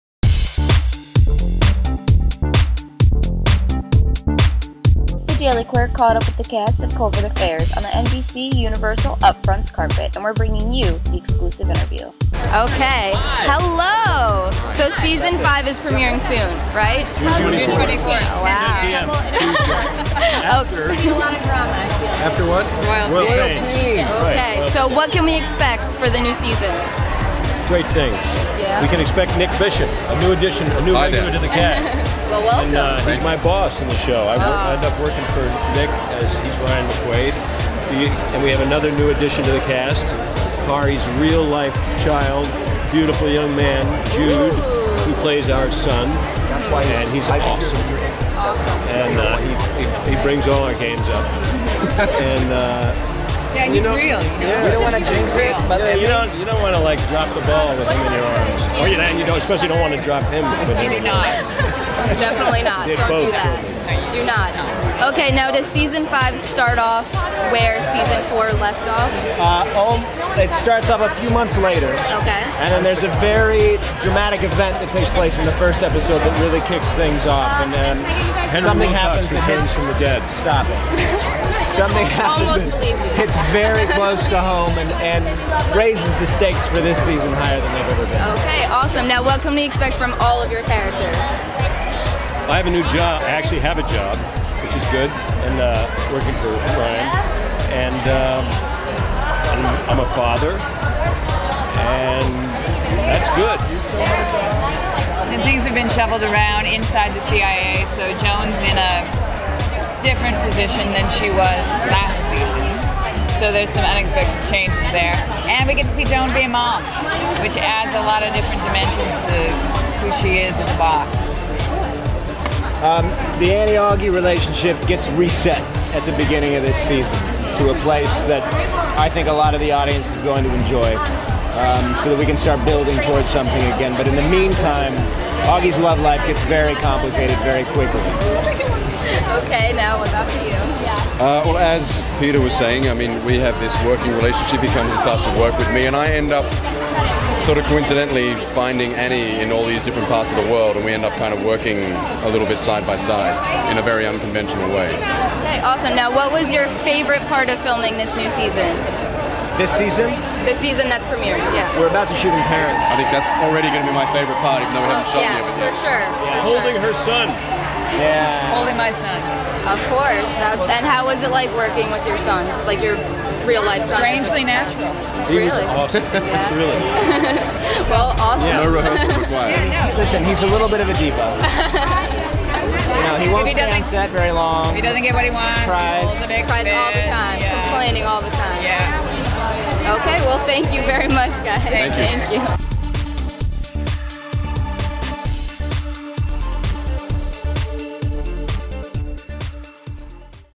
An Exclusive Interview with the cast of ‘Covert Affairs’
The Daily Quirk caught up with Covert Affairs stars Peter Gallagher, Kari Matchett, Christopher Gorham and new cast member Nic Bishop on the NBCUniversal Cable Upfronts red carpet where they shared details about the show's upcoming Season 5! Watch the exclusive interview below to find out where Season 5 picks up after the shocking Season 4 finale, about the the new cast members joining the show and much more… Read More